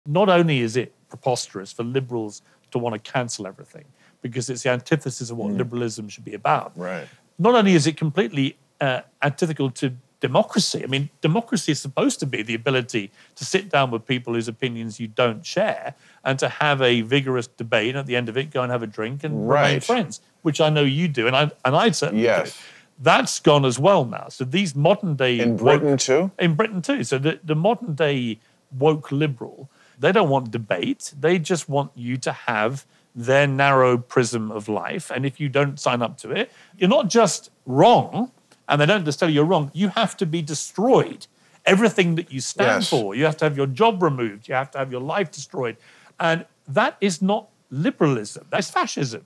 Cancel Culture is FASCISM w/ Piers Morgan
Bill Maher and Piers Morgan randomly riff on Fascism